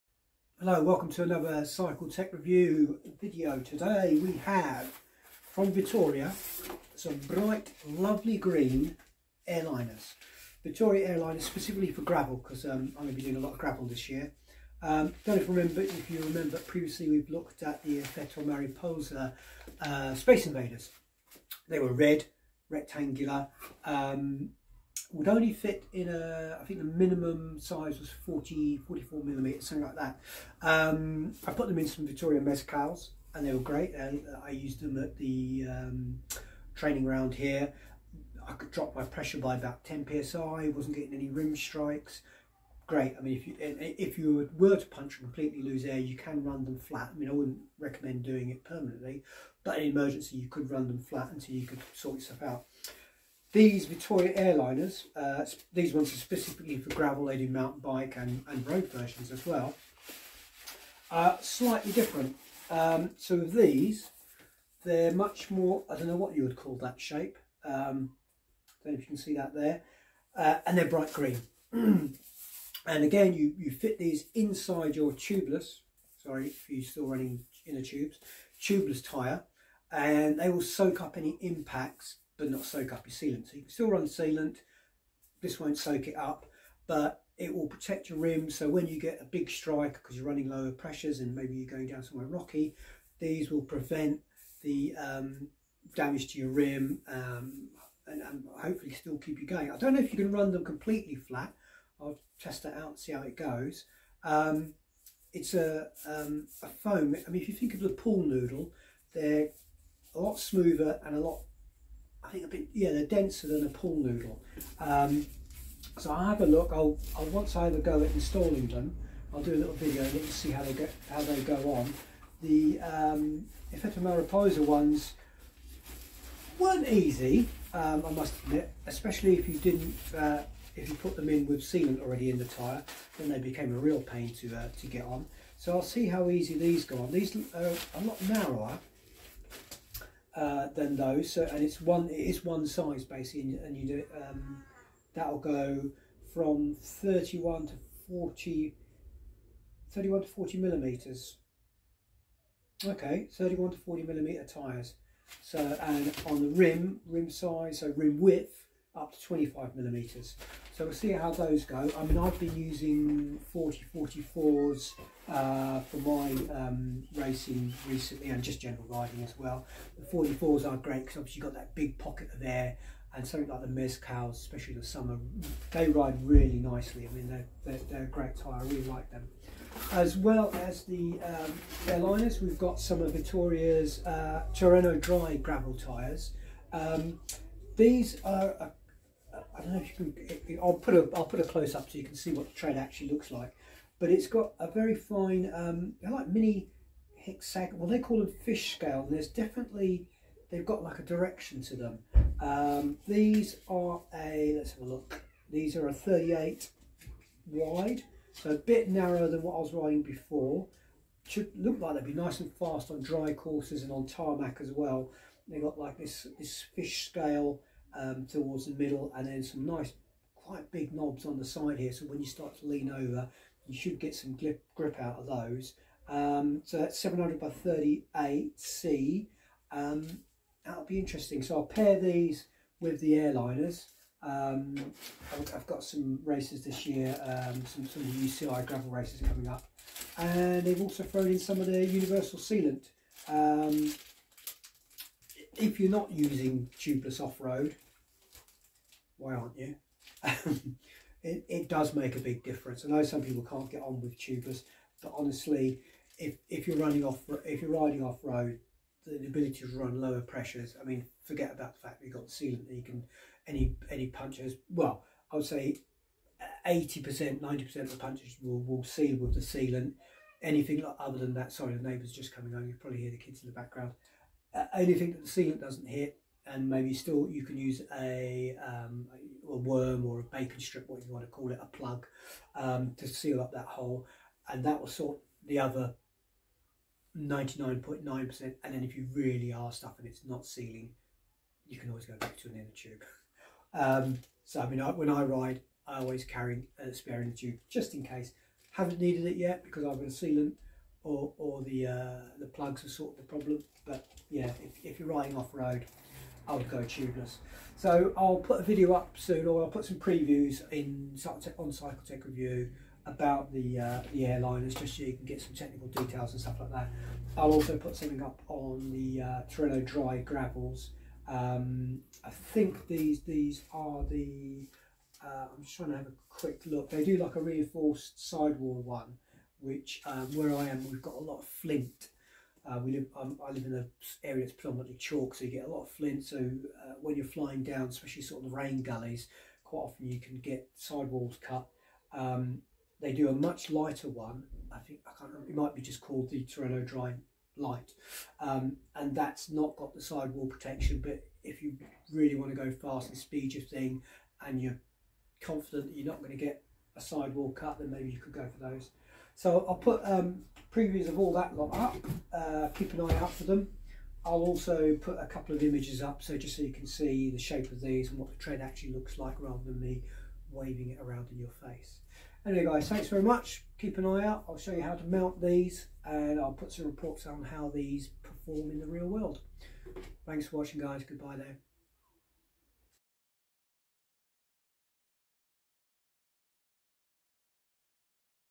Vittoria Airliners Gravel and Dry Gravel Endurance tyres
I take a look at Vittoria's Airliner Gravel tyre insert and Terreno Dry Endurance tyres.

The inserts are designed specifically for gravel, the MTB and road versions have different profiles and sizes. Inserts are great for protecting your rims and preventing snake-bite punctures when you hit sharp edges. The foam soaks up the impact and lets you run lower pressures, which increases comfort and grip.

The Terreno Dry Gravel Endurance tyres are one of three versions of this Dry Gravel tyre, which all share the same tread pattern. The Endurance version has added sidewall protection and comes in 37, 40 and 45mm widths for 700c.

Once the terrain dry outs I'll fit the tyres and see how they perform, but while I'm waiting for that to happen, the Airliners can go in my winter tyres to see how they perform.